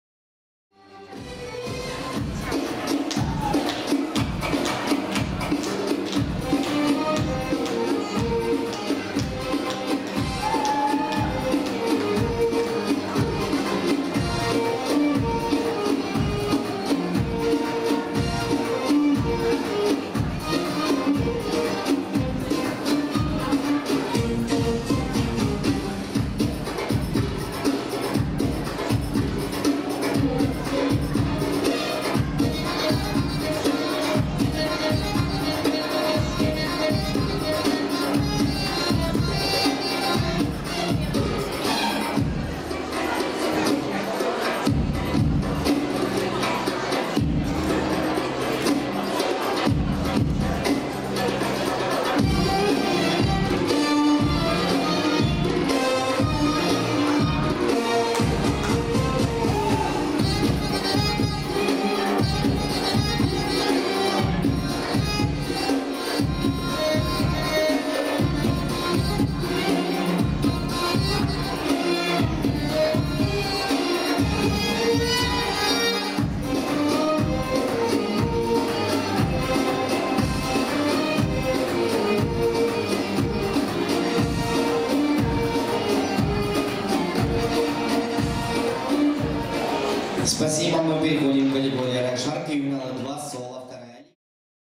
Polina Hadzhinova ⊰⊱ Oriental Constellation '14.
Полина Хаджинова на конкурсе фестиваля «Созвездие Востока 2014».
02.11.14. Днепропетровск. ул. Шолом-Алейхема, 4/26. Центр «Менора»
(Raqs el sharki Ювеналы 1 Соло 2 Лига)
 3,44